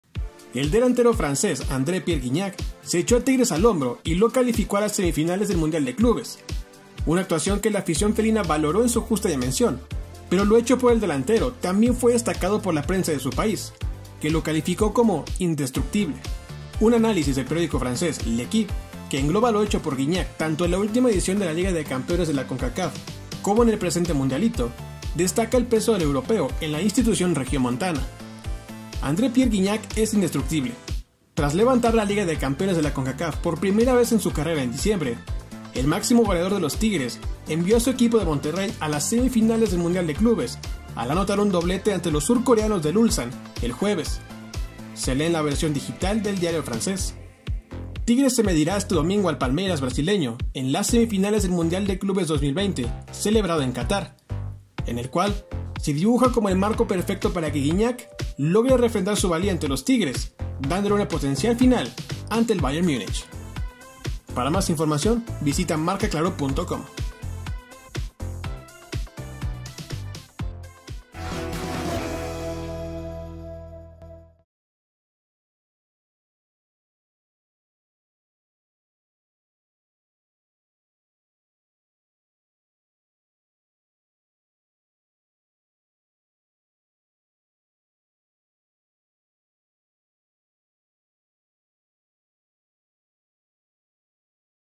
André Pierre Gignac, el "indestructible" de los Tigres
Los Tigres están en la antesala de la gran final del Mundial de Clubes y en gran medida se debe al francés, André-Pierre Gignac, quien fue destacado por el diario L'Equipe de su país por ser "indestructible".

El doblete de Gignac en el partido del Ulsan, que le dio el derecho al cuadro universitario de jugar la semifinal ante el Palmeiras de Brasil, donde el francés volverá a fungir como el hombre más importante del club.